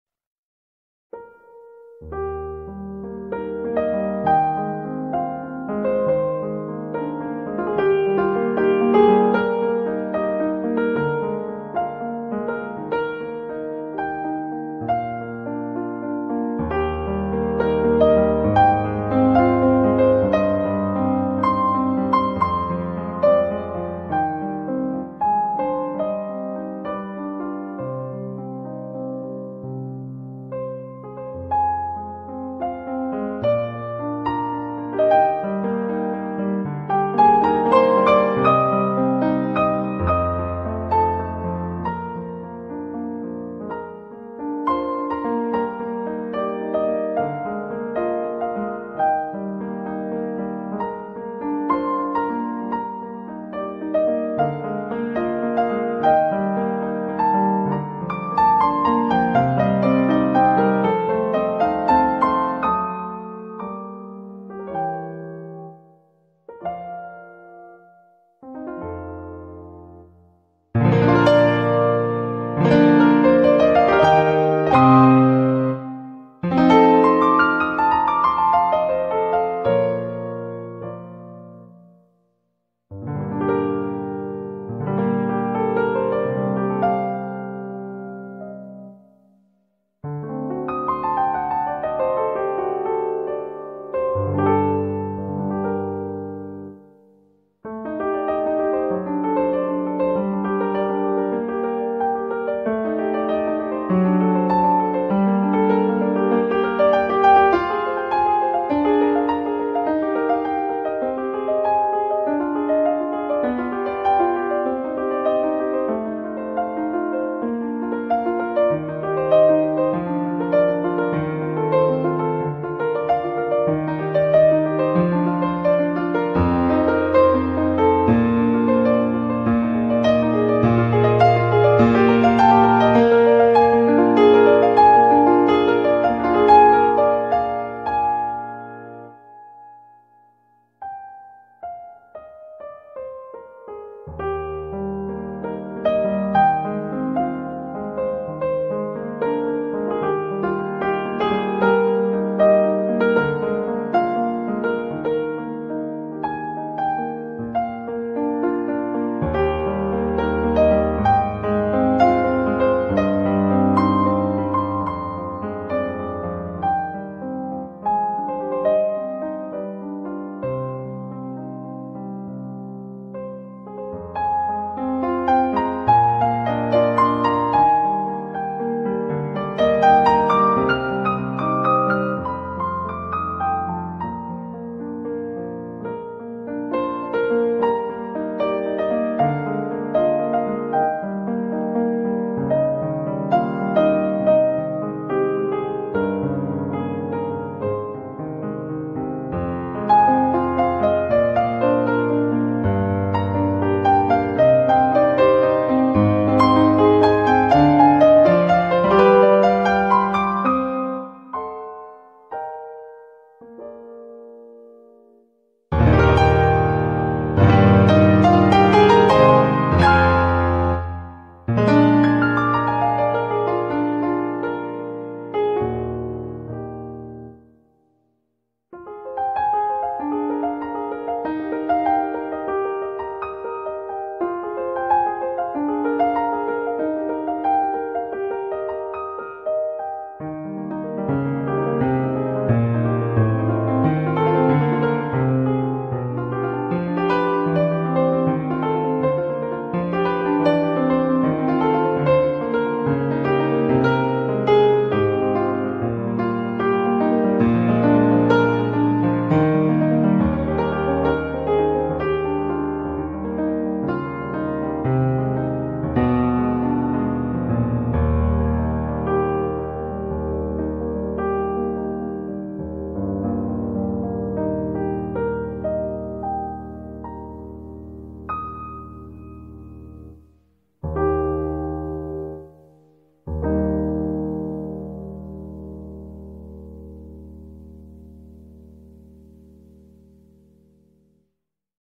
Frederick Carl Frieseke
Frederick Carl Frieseke a peint tout au long de sa vie des personnages féminins. Les femmes de l'artiste sont souvent rêveuses, toujours élégantes et un rien distantes.

                   Mikhail Ivanovich Glinka
Piano      : Sam Chan